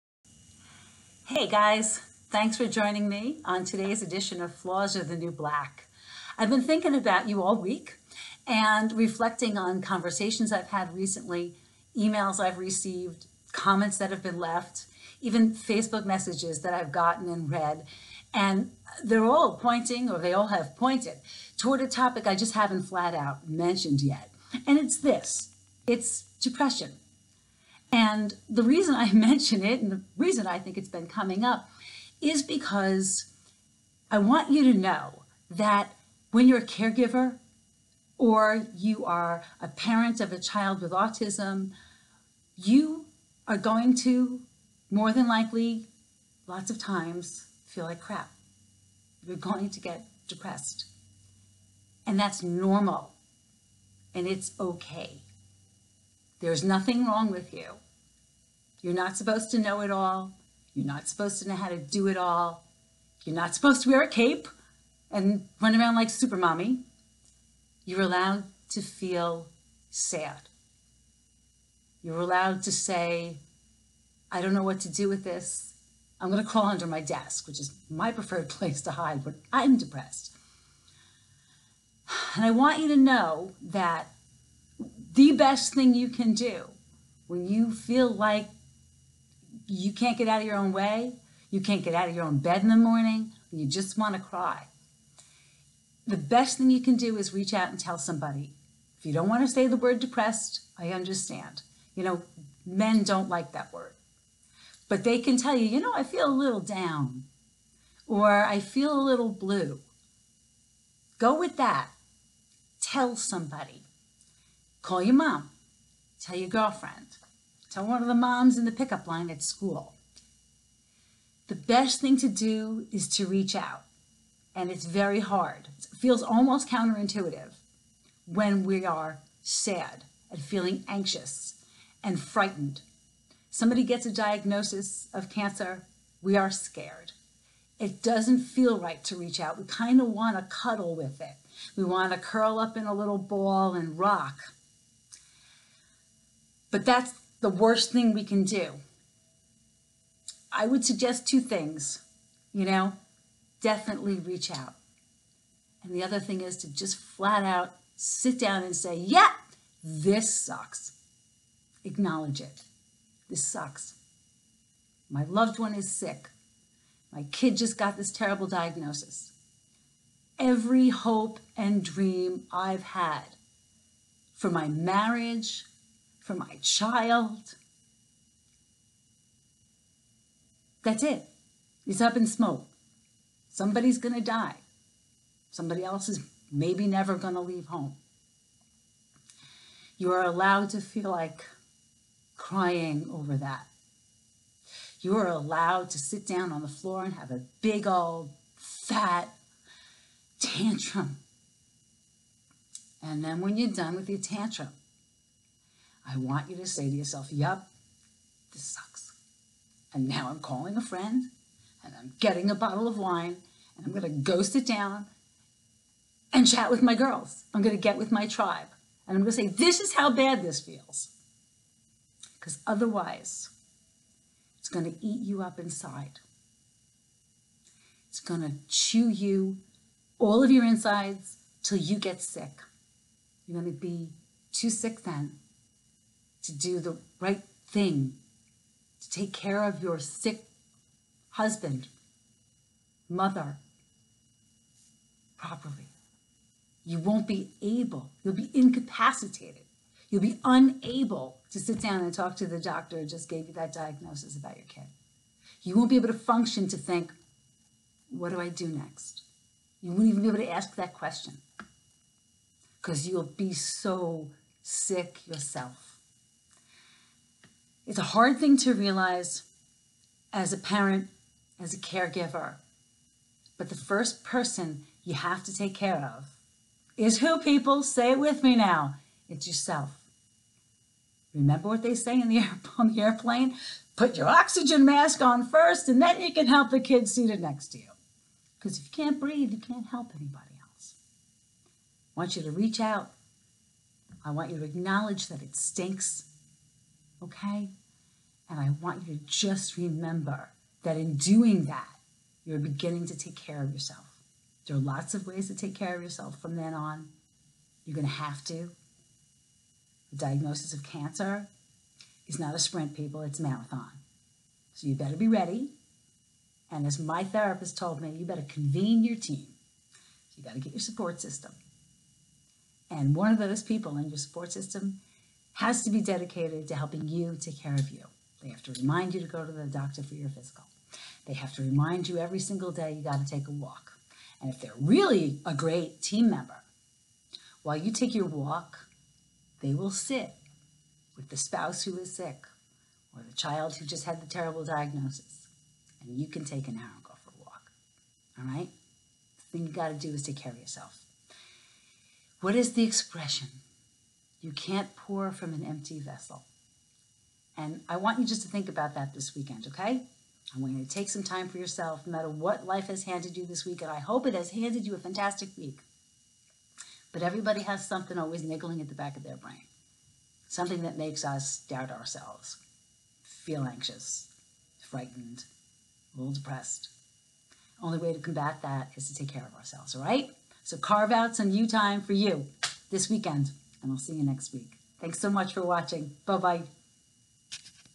Flaws Are The New Black presents Depression 101: This Sucks
The doctors found a tumor. Your spouse wants a divorce. Your kid just got diagnosed with autism. Life throws us some devastating curve balls that can result in depression. What to do? I'm going to give you a couple of tips in Depression 101: Start by saying "This sucks!"